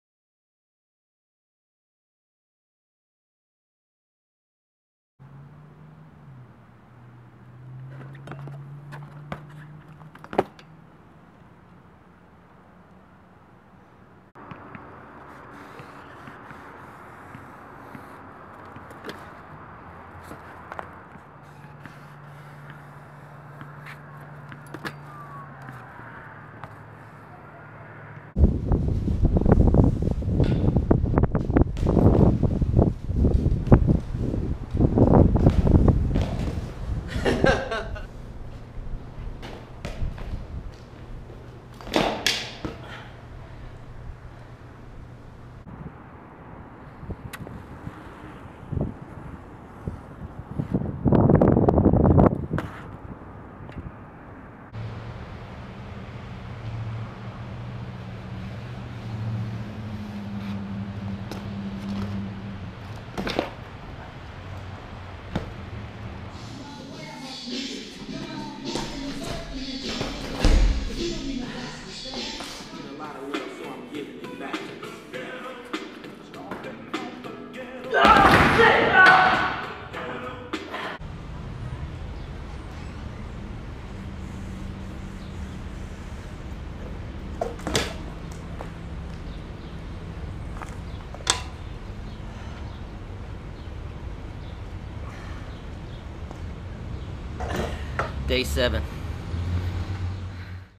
Seven days seven new tricks challenge
I decided to challenge myself do pull 7 new tricks in seven days and post them each day. Here's all 7.